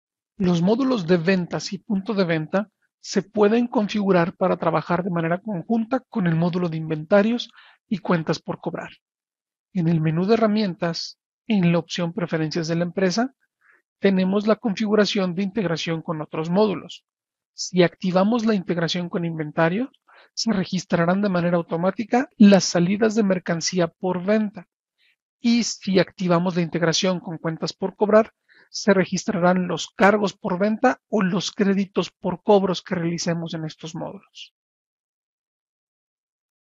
Microsip Punto de Venta: Cómo configurar integración con Inventarios y Cuentas por cobrar
*Esta acción también la puedes hacer desde el módulo de Ventas*

Microsip es el ERP que se adapta a tu empresa.